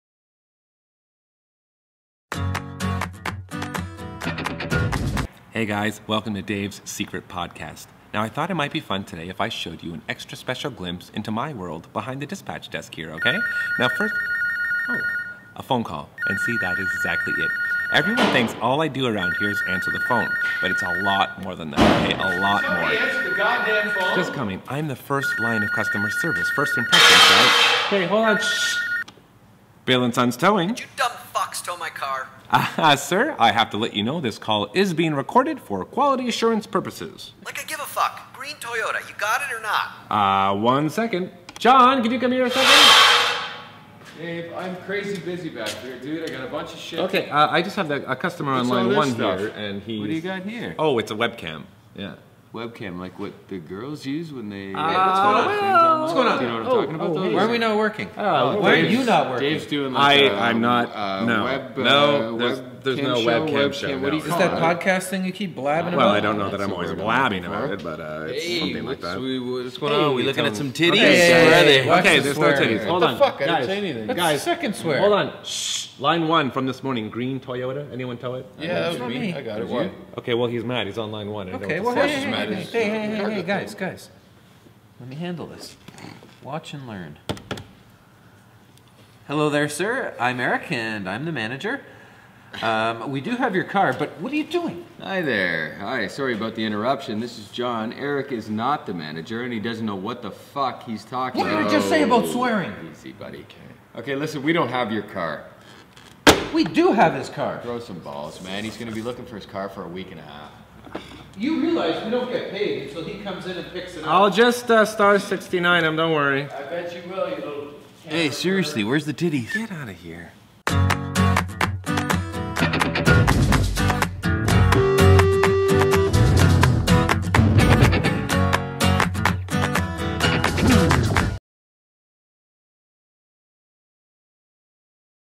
Welcome to Bill & Sons Towing! - Dave's Secret Podcast
- In the latest episode of Dave's Secret Podcast, Dave Vanderchuck takes you behind the scenes of his job at the garage.

starring The Imponderables
written by Mark De Angelis
directed by Simon Fraser

BILL & SONS TOWING
Bros Before Tows